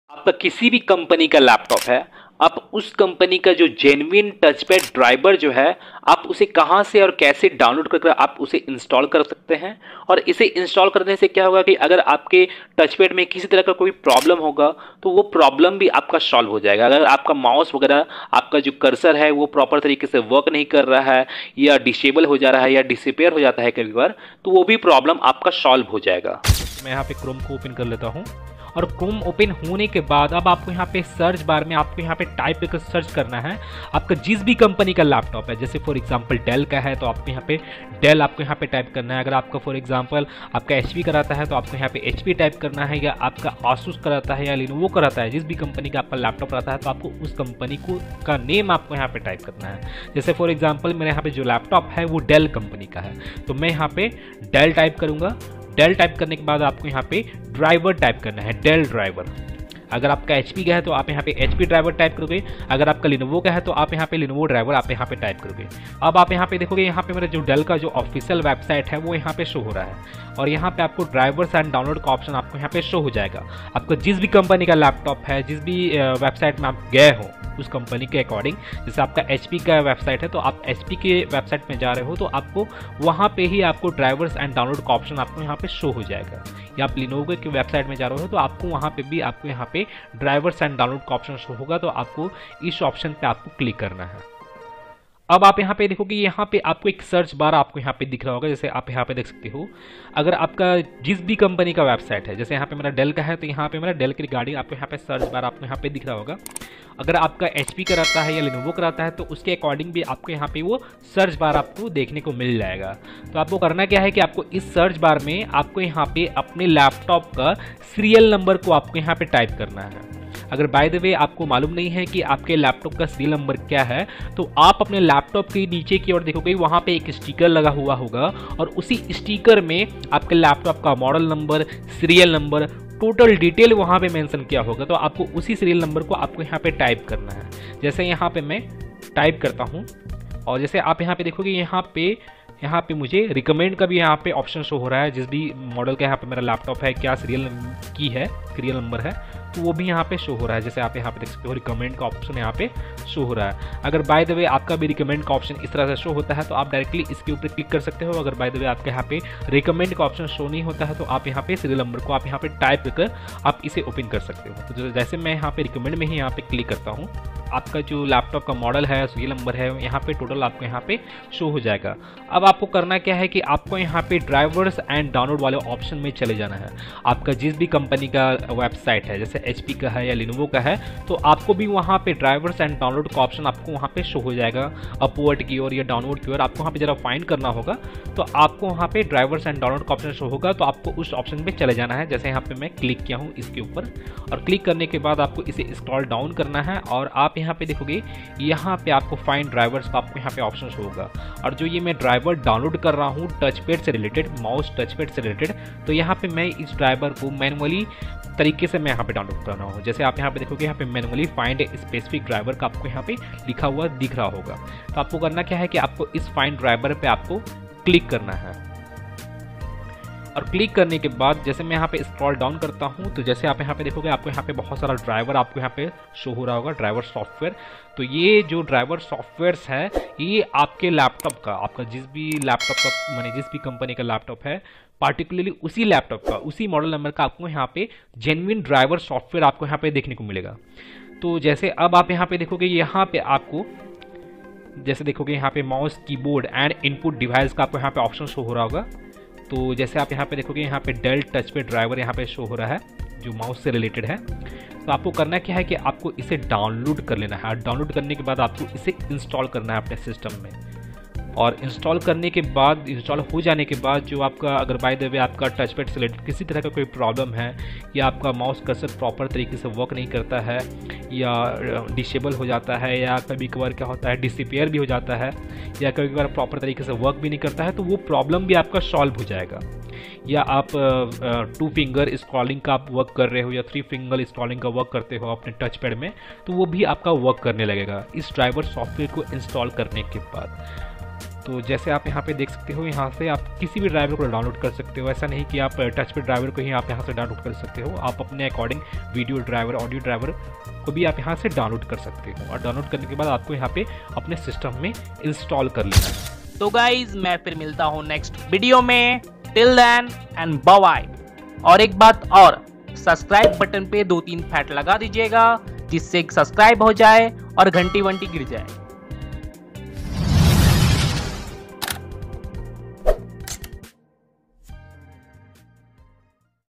Touchpad Not Working in Windows 11/10? Here's the Fix! (100% Working)
🖱️ Kya aapka laptop ka touchpad Windows 11 ya 10 me kaam nahi kar raha? Tension mat lo bhai! Iss video me maine step by step bataya hai kaise aap touchpad not responding problem ko easily fix kar sakte ho.

🎯 Ye method kaam karega har laptop brand pe:
HP, Dell, Lenovo, Acer, Asus, etc.

📌 Video ko dhyan se pura end tak dekho aur har step follow karo.
Agar aapke koi questions hain, toh comment section mein zaroor batayein ));👍👍